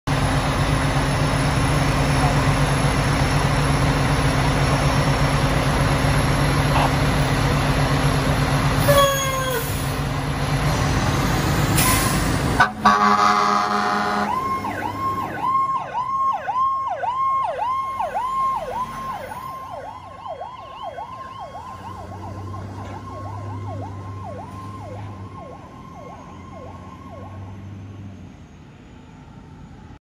Mill Creek 21-5 & 21-7 resounding to assist Elsmere on a structure dispatch.
21-6 and 21-7 respond. Cancelled en route.